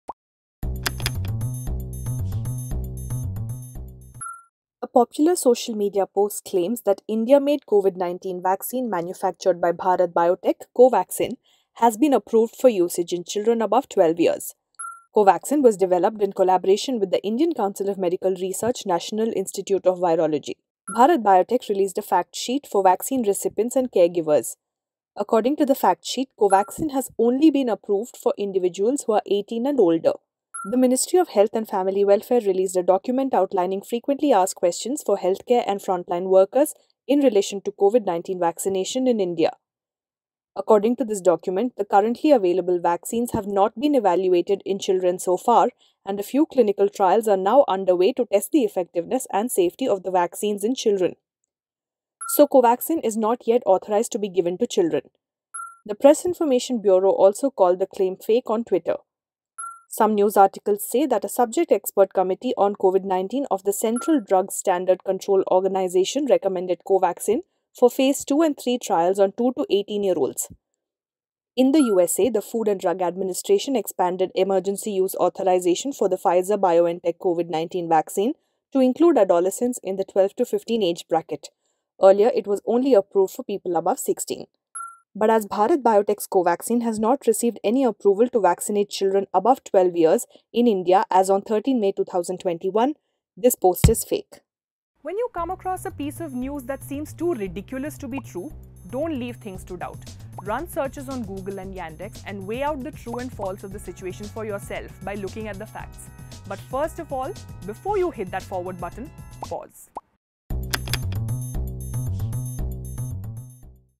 FACT CHECK: Has Bharat Biotech's Covaxin been Approved for 12+ Children? || Factly
A popular social media post claims that India-made COVID-19 vaccine Covaxin may now be used to inoculate children aged 12 and above. We check the veracity of the claim in this video.